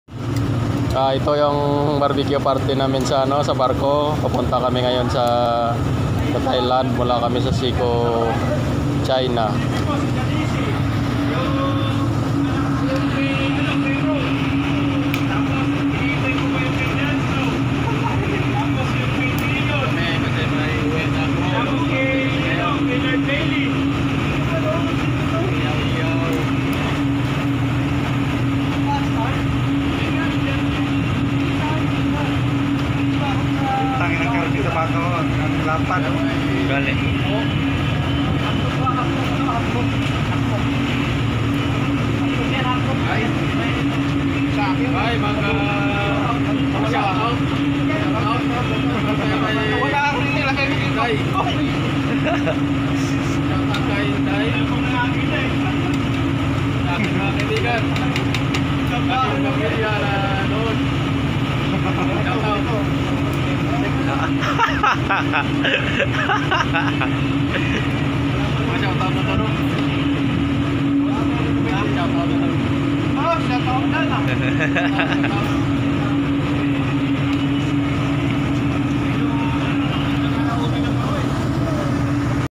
Barbeque Party in The Ship during Vessel Underway @ Sea
Barbeque Party in the Ship during vessel at Sea.